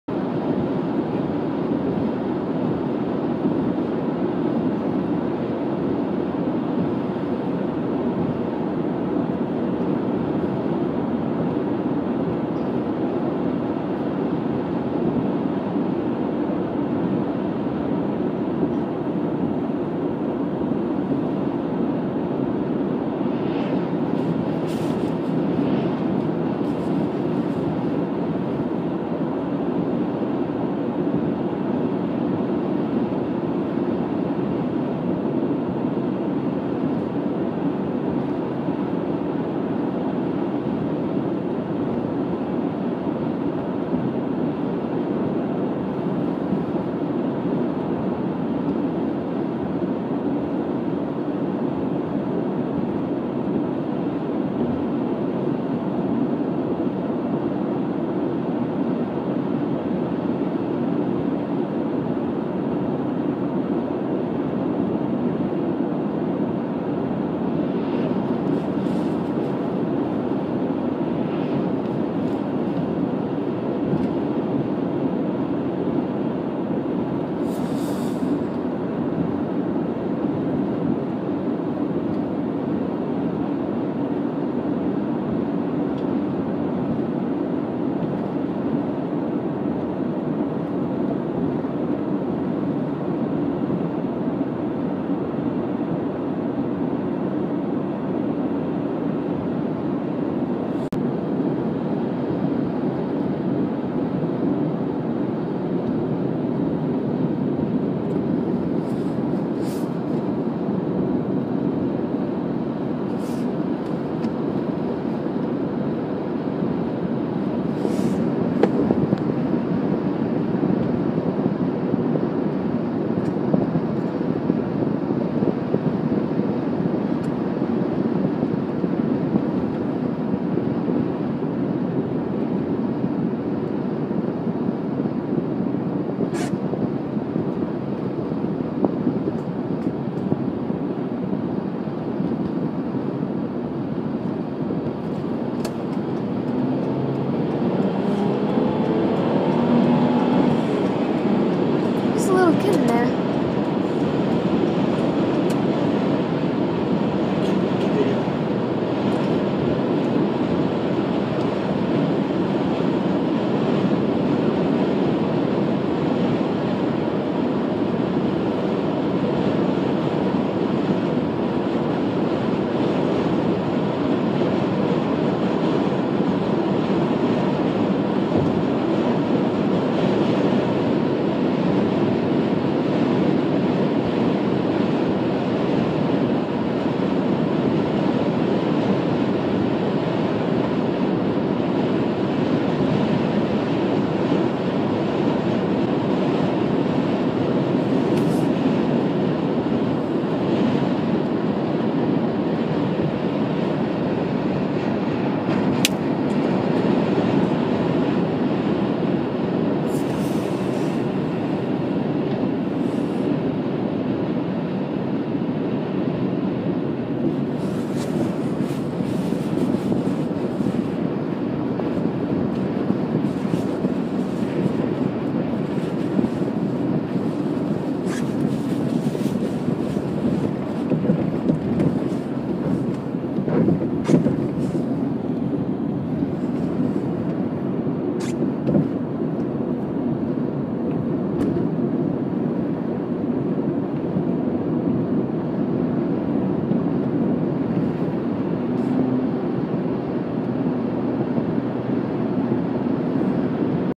Hwy 12 East Bound - Monitor, Alberta
Just before and after Monitor, Alberta. Deteriorating road conditions at approximately 5:45 pm on Thursday, Mar 21, 2013. Road conditions pristine an hour previous. Video shot by my daughter, Ashlyn McCormick, on her iPod Touch.